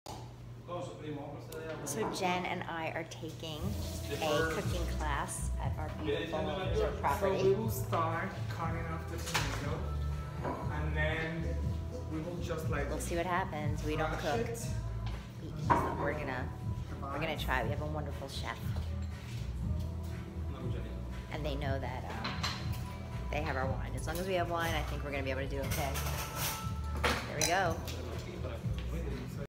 dominicancookingclass
Staying at the luxury resort In the Dominican Republic, Cass Del XVI we had to learn how to cook some amazing food with their executive chef,